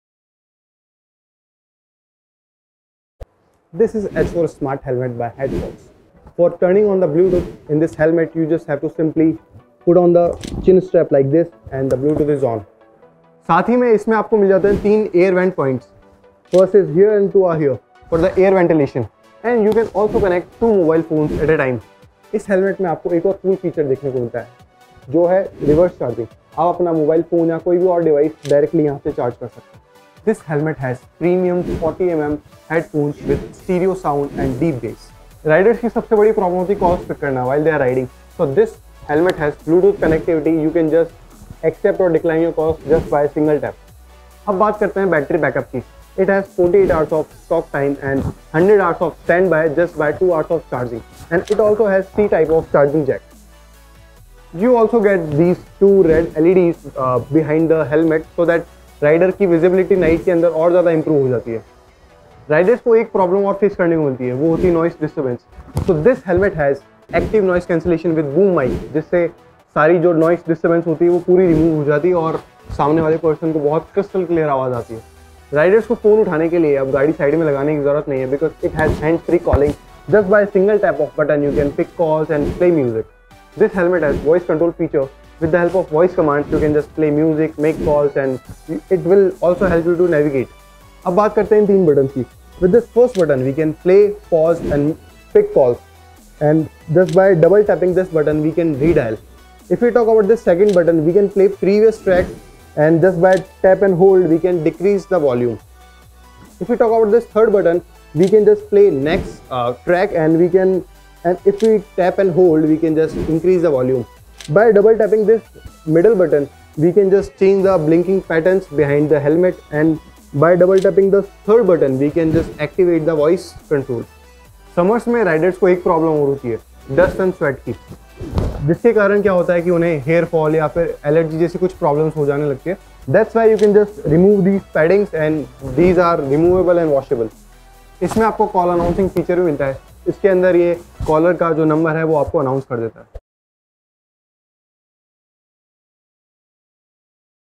How to use Headfox H4 model - Video guide, Applicable on all H4 Models
The Headfox Smart Bluetooth helmet uses cutting-edge DSP echo cancellation and noise suppression technology to deliver crystal-clear music and voice calls at lightning-fast speeds.

Details

Voice control for helmets Bring me to the nearest petrol station, make a call to Dad, and turn on the music.
With the 5.2 Bluetooth Smart Helmet Technology, you can enjoy music, GPS, and voice control while riding.
Two high-quality headphones with exceptionally crisp audio stereo sound are included.
Extended-Life Battery 48 hours of talk time and 110 hours of standby time are achieved after 2 hours of full charging.
The upper air intake vent is positioned to increase air volume into the interior of the helmet, while the back exit vent is in the best flow-through ventilation position.
 Advance Features

Packed with Smart Features Advance automatic ON/OFF system when you tighten your chinstrap by wearing a helmet it will automatically turn ON Bluetooth and when you open chinstrap the Bluetooth will automatically turn OFF.
3 Button Control Helmet supports hands-free call answering, rejecting incoming calls and redialing last.
Advance Power bank for mobile phone charging and other devices.
Important Features

It is recommended to use breathable padding with a neck protector and additional comfort on long rides. Clean interior with an Italian multi-pore design for better ventilation throughout the summer.
It is possible to remove and clean all internal padding.